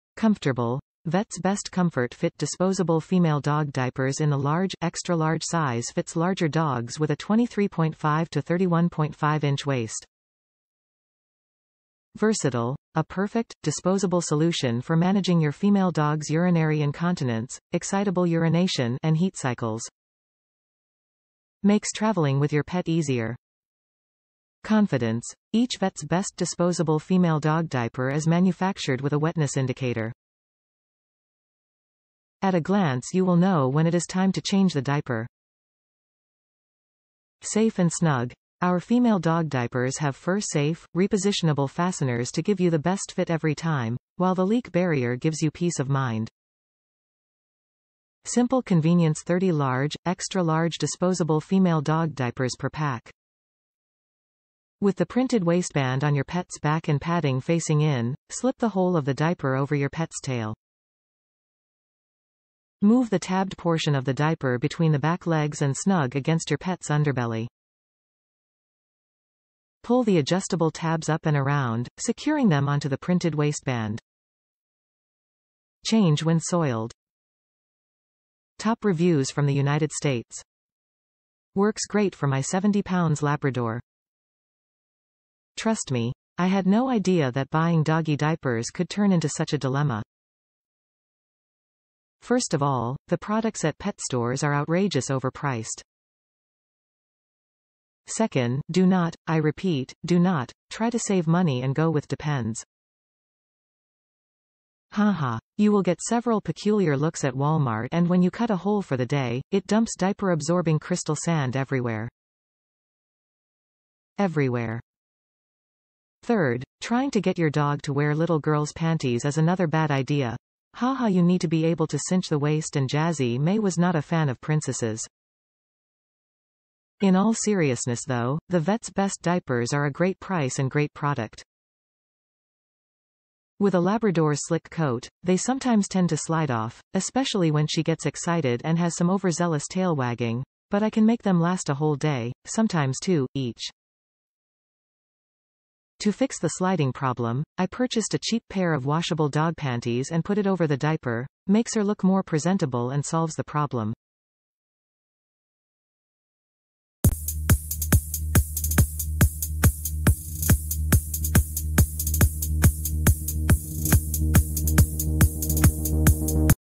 Vet's Best Comfort Fit Dog Diapers | Disposable Female Dog Diapers | Absorbent with Leak Proof Fit
Comfortable - Vet's Best comfort-fit disposable female dog diapers in the large/extra large size fits larger dogs with a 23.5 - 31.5 inch waist.
Versatile - a perfect, disposable solution for managing your female dog’s urinary incontinence, excitable urination, and heat cycles. Makes traveling with your pet easier.
Confidence - Each Vet’s best disposable female dog Diaper is Manufactured with a wetness indicator. At a glance you will know when it is time to change the Diaper.
Safe and snug - Our female dog diapers have Fur-safe, repositionable fasteners to give you the best fit every time, while the leak barrier gives you peace of mind.
Simple convenience - 30 large/extra large disposable female dog diapers per pack.
With the printed waistband on your pet's back and padding facing in, slip the hole of the diaper over your pet's tail. Move the tabbed portion of the diaper between the back legs and snug against your pet's underbelly. Pull the adjustable tabs up and around, securing them onto the printed waistband. Change when soiled.

 Works Great for my 70 lb Labrador!
Trust me - I had no idea that buying doggie diapers could turn into such a dilemma. First of all, the products at "Pet Stores" are outrageous overpriced. Second, DO NOT, I repeat, DO NOT, try to save money and go with Depends. Haha! You will get several peculiar looks at Walmart and when you cut a hole for the day, it dumps diaper absorbing crystal sand everywhere. EVERYWHERE. Third, trying to get your dog to wear little girls panties is another bad idea - Haha you need to be able to cinch the waist and Jazzy May was not a fan of Princesses.

In all seriousness though, the Vet's Best diapers are a great price and great product. With a labradors slick coat, they sometimes tend to slide off (especially when she gets excited and has some overzealous tail wagging) but I can make them last a whole day (sometimes two) each. To fix the sliding problem, I purchased a cheap pair of washable dog panties and put it over the diaper - Makes her look more presentable and solves the problem.